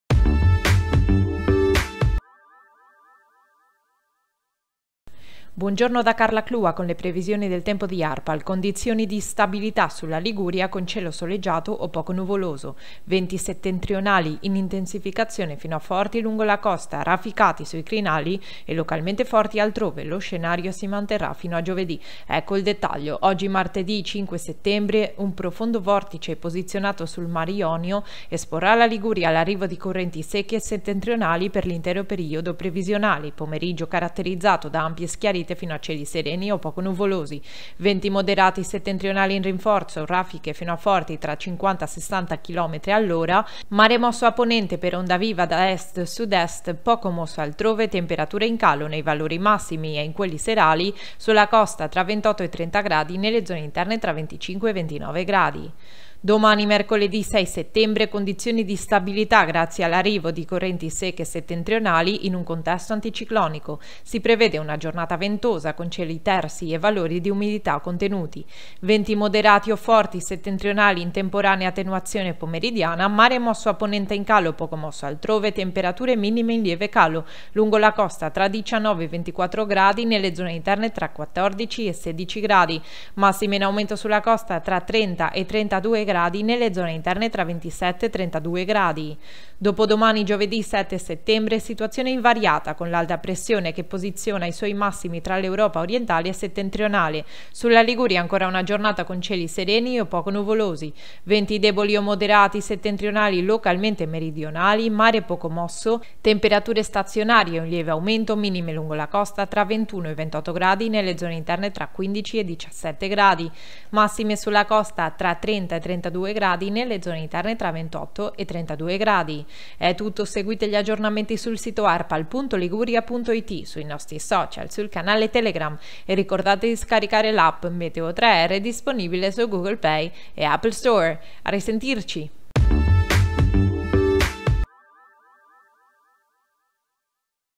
5 settembre 2023, le video previsioni di Arpal
Condizioni di stabilità sulla Liguria con cielo soleggiato o poco nuvoloso. Venti settentrionali in intensificazione fino a forti lungo la costa, rafficati sui crinali e localmente forti altrove. Lo scenario si manterrà fino a giovedì.